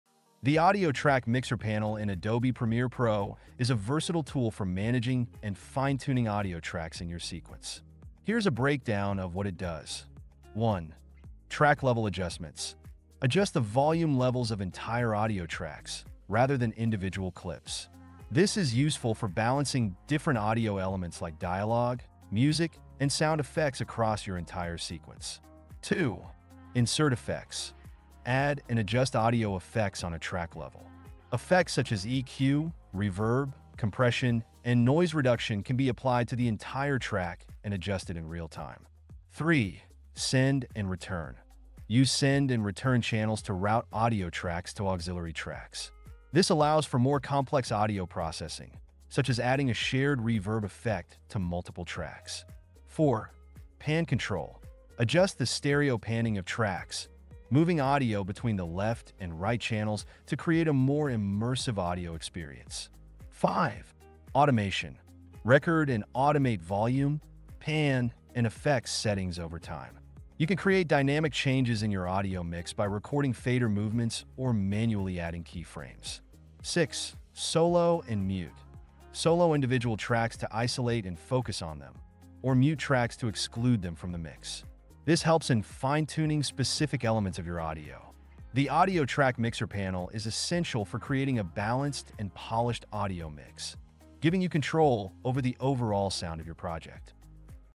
The Audio Track Mixer Panel In Adobe Premiere Pro 2025.
In this tutorial, we're diving into the Audio Track Mixer Panel in Adobe Premiere Pro. Discover how to adjust track volumes, apply real-time effects, and manage your audio channels like a pro. We'll guide you step-by-step to help you achieve a professional and immersive sound mix.